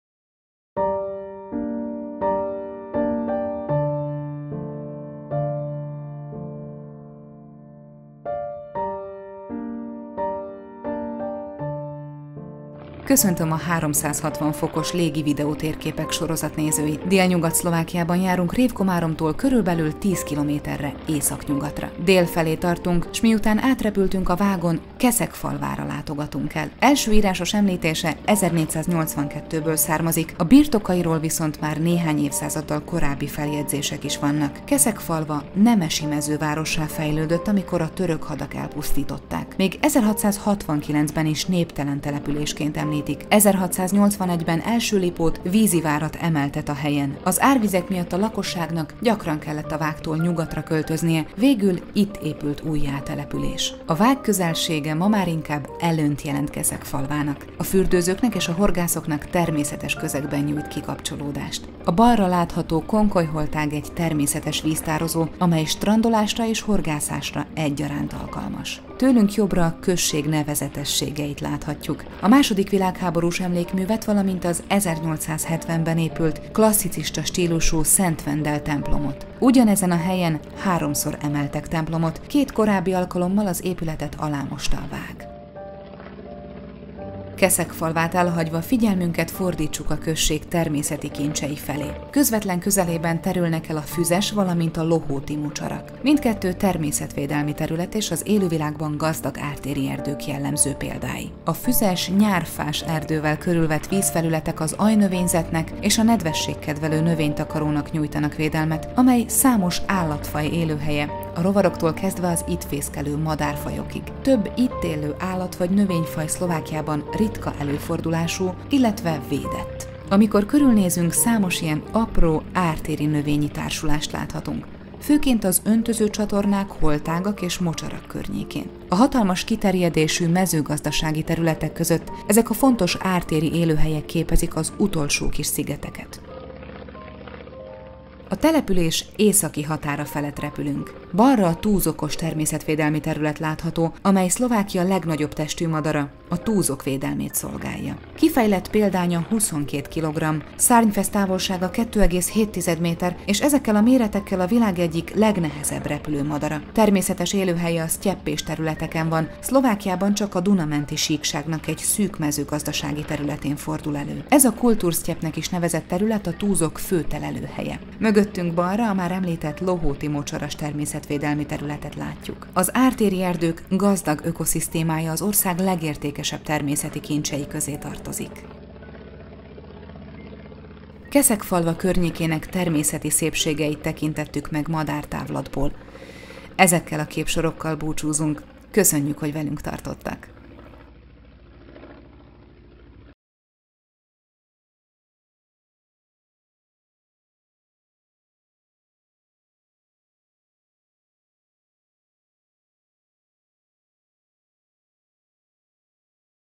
360°- os videótérkép Keszegfalva – madártávlatból
Pilóta, operatőr, rendező: Milan Paprčka
Animációs grafika, vágó: Mgr. art. Zuzana Maďarová
Szöveg: Ing. Matej Schwarzbacher
Narrátor: PhDr. Récsei Noémi
Fordító: Mgr. Hizsnyai Tóth Ildikó, PhD
Zene: Lance Conrad